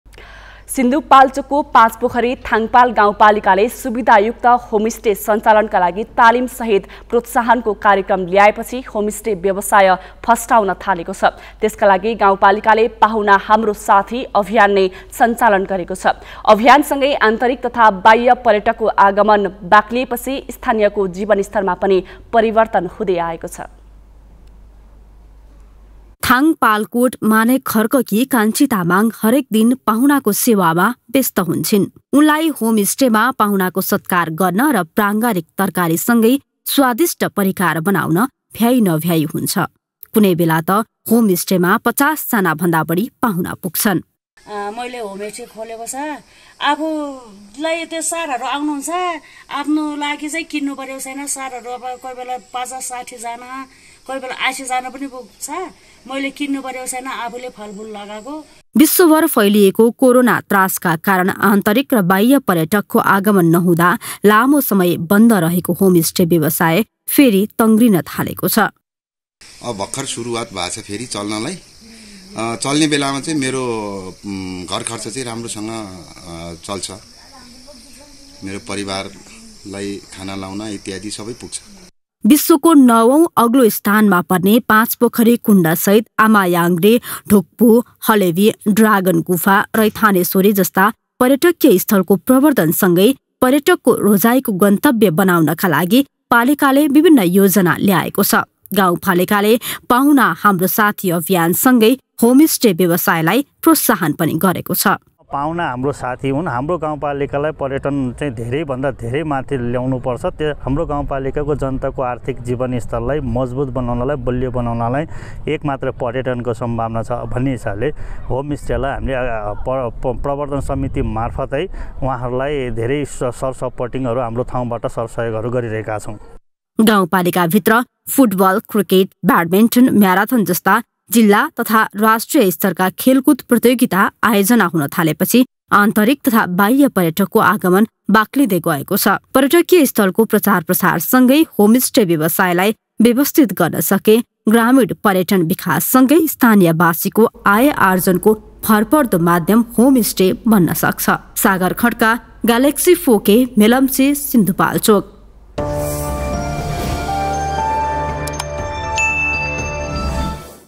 होमस्टे व्यवसाय फस्टाउदै | Galaxy News Report
सिन्धुपाल्चोकको पाँचपोखरी थाङपाल गाउँपालिकाले सुविधायुक्त होमस्टे सञ्चालनका लागि तालिमसहित प्रोत्साहनको कार्यक्रम ल्याएपछि होमस्टे व्यवसाय फस्टाउन थालेको छ । त्यसका लागि गाउँपालिकाले ‘पाहुना हाम्रो साथी’ अभियान नै सञ्चालन गरेको छ । अभियानसँगै आन्तरिक तथा बाह्य पर्यटकको आगमन बाक्लिएपछि स्थानीयको जीवनस्तरमा पनि परिर्वतन हुँदै आएको छ ।